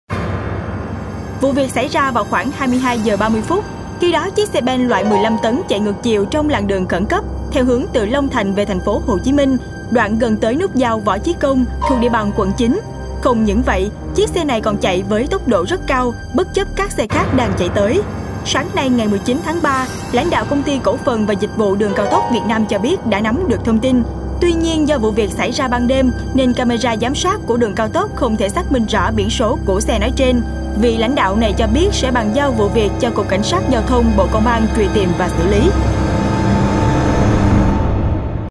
HOẢNG HỒN VỚI XE BEN CHẠY NGƯỢC CHIỀU TRÊN CAO TỐC
Kinh hoàng 1 chiếc xe ben chạy ngược chiều trên đường cao tốc TPHCM Long Thành Dầu Giây tối qua (18/3) khiến các xe lưu thông đúng chiều 1 phen hoảng sợ!
Lỗi đi ngược chiều trên đường cao tốc sẽ bị phạt 7-8 triệu đồng, đồng thời tài xế bị tước bằng lái xe 4-6 tháng!